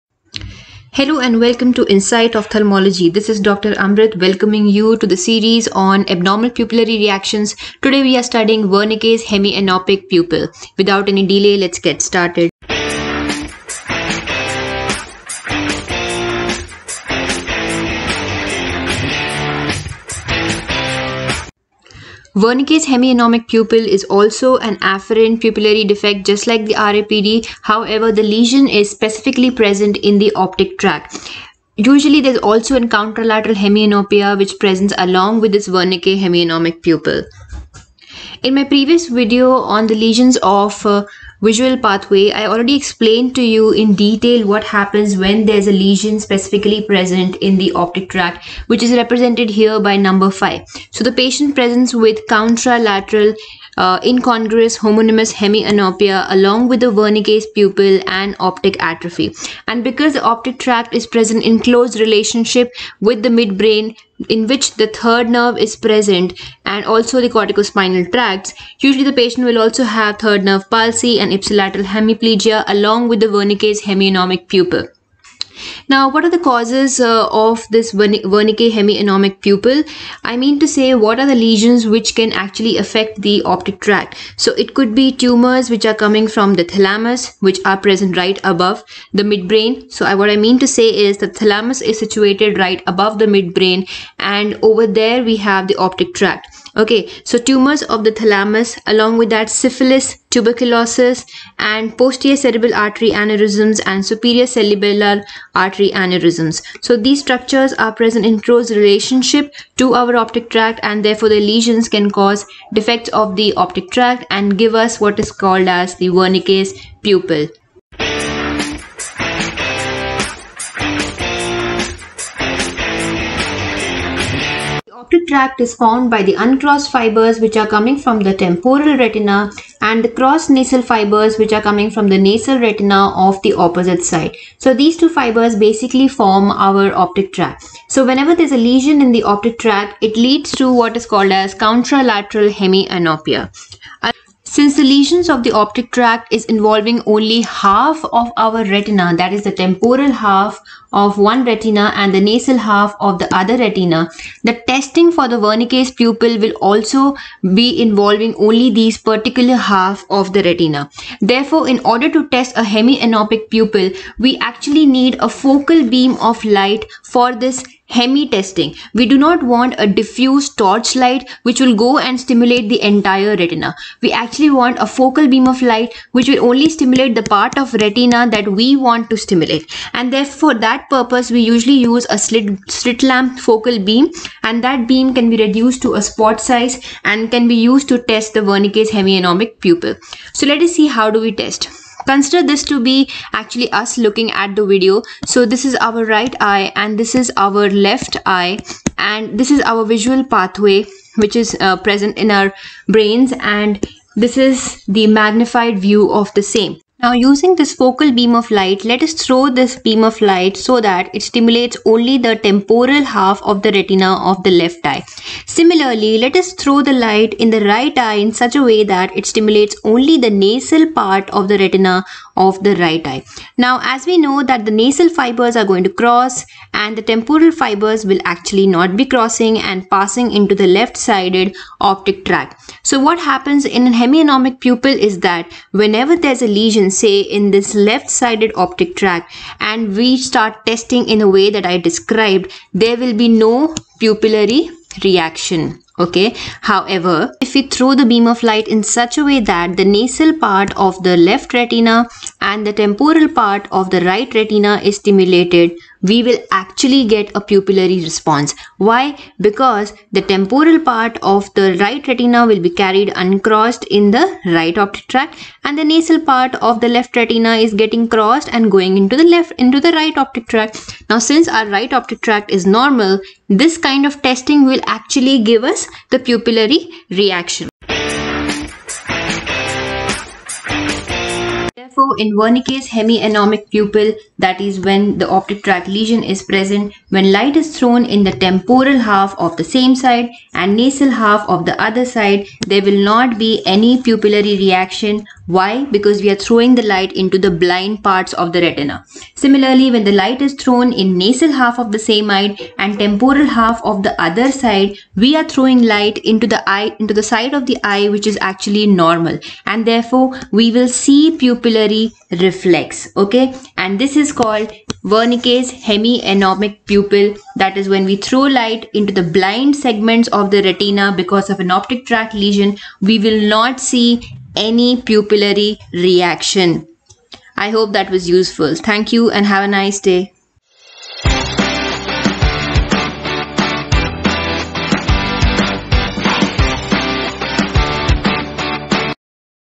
WERNICKE'S HEMIANOPIC PUPIL || Abnormal pupillary reaction series #2
WELCOME TO INSIGHT OPHTHALMOLOGY

This video is a part of the series on abnormal pupillary reactions. In the previous part of the series we discussed Marcus Gunn pupil. In this part we discuss the Wernicke's Hemianopic pupil. From basics to advanced !!
I hope you enjoy our videos

LINKS :-

Abnormal pupillary reaction #3 ( ARGYL ROBERTSON PUPIL )

Abnormal pupillary reaction#5 (Understanding Light Near Dissociation)

TIMESTAMPS
00:09 What is Wernicke's Hemianopic pupil
01:58 How to test Wernicke's pupil
04:37 Interpretation and diagnosis of the Wernicke's pupil